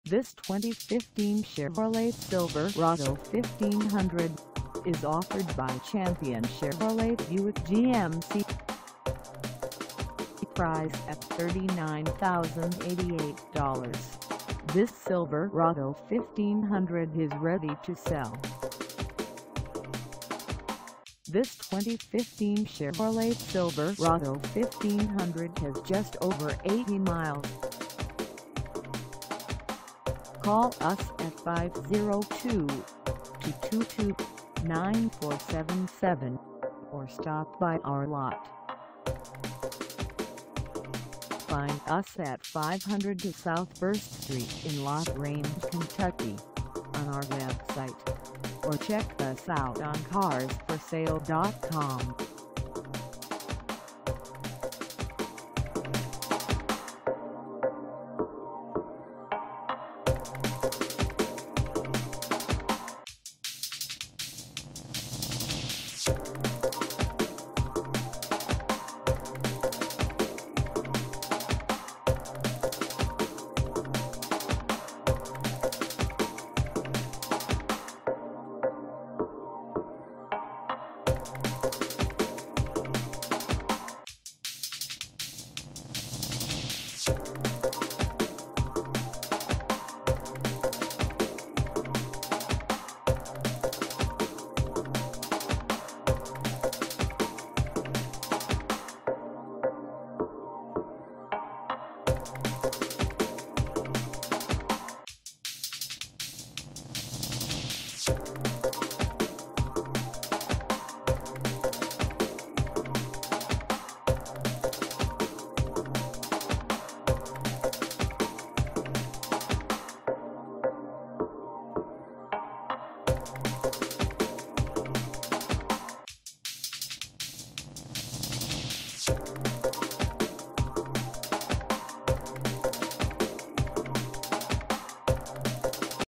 2015 Chevrolet Silverado 1500 LT for sale in La Grange, KY 4
This 2015 Chevrolet Silverado 1500 LT is for sale in La Grange, KY 40031 at Champion Chevrolet Buick GMC.
Engine: V8 5.3L V8
Transmission: Automatic 6-Speed
Color: Deep Ocean Blue Metallic
Mileage: 80
Contact Champion Chevrolet Buick GMC 502 South 1st Street La Grange, KY 40031 to test drive this 2015 Chevrolet Silverado 1500 LT today.